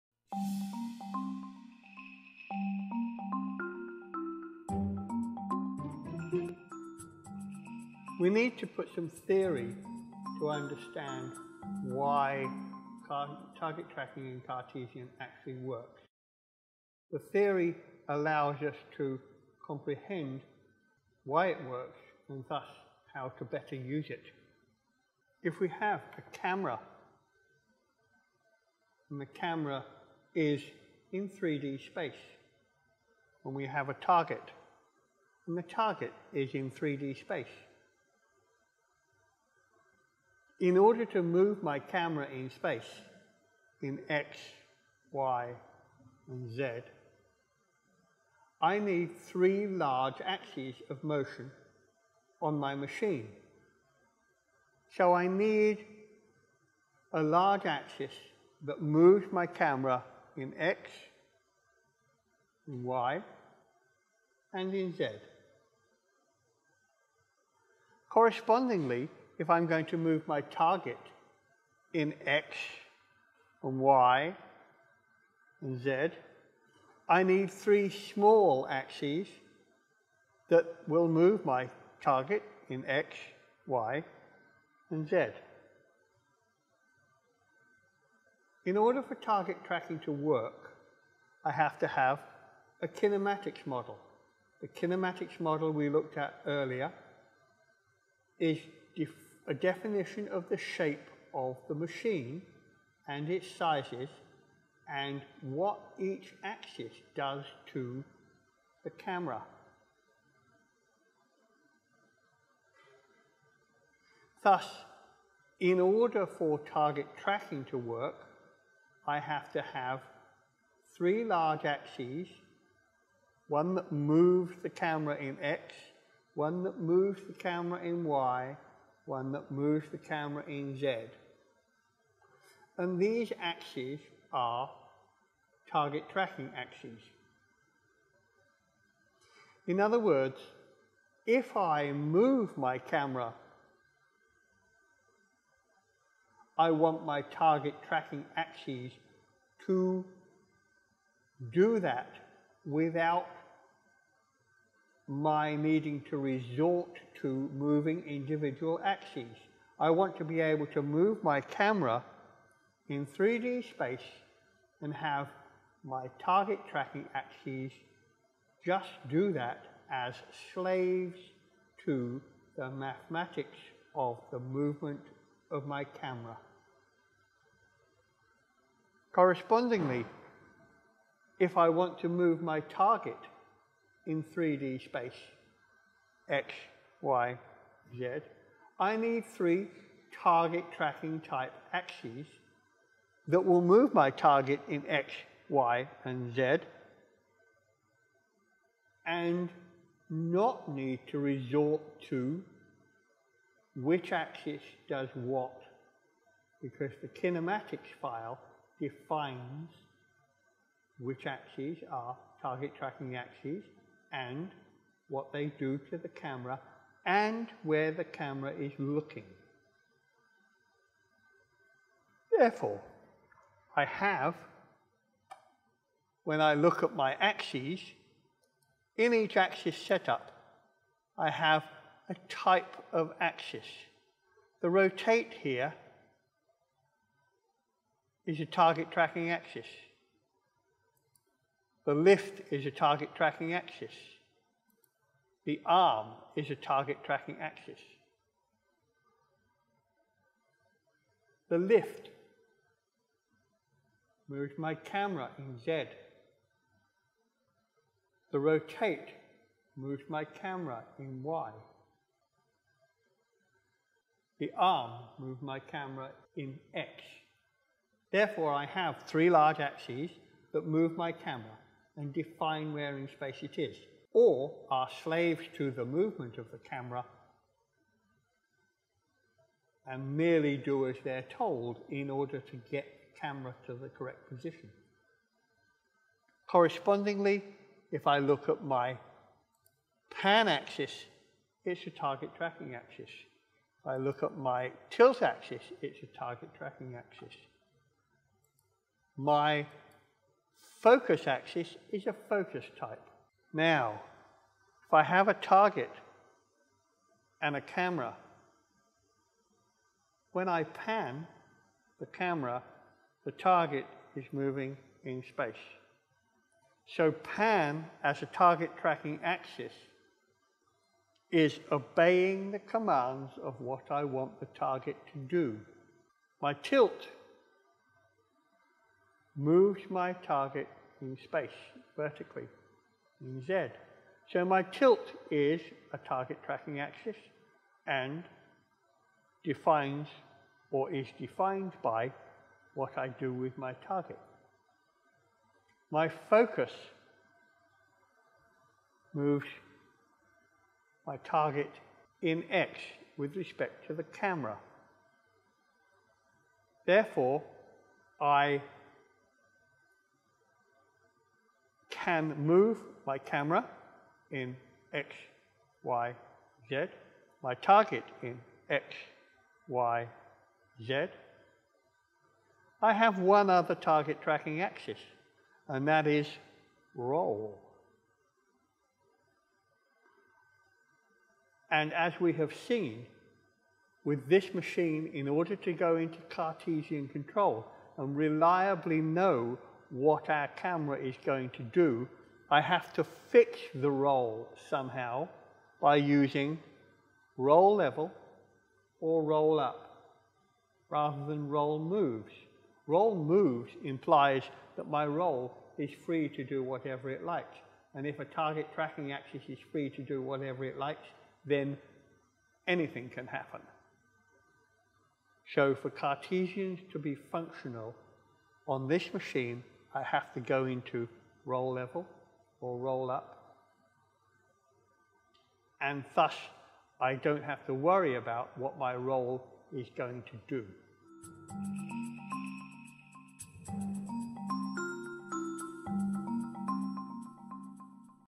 09 - TT and cart theory
Get a better understanding of cartesian control and it’s relation to target tracking.